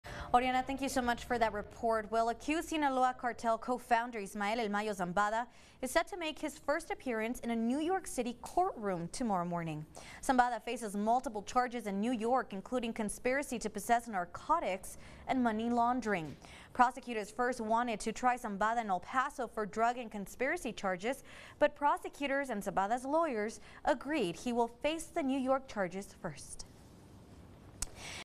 ‘El Mayo’ Zambada set to appear in New York courtroom
Accused Sinaloa cartel co-founder Ismael “El Mayo” Zambada is scheduled to make his first appearance in a New York City courtroom on Friday morning.